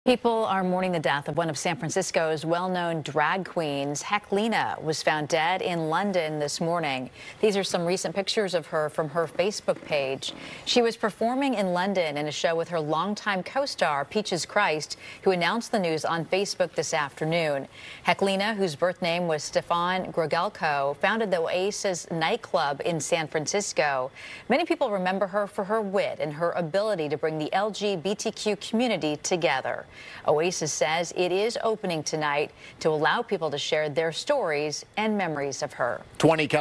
San Francisco mourns loss of legendary drag queen, Heklina
Reactions are pouring in upon the news of legendary San Francisco drag queen, Heklina's sudden passing in London on Monday.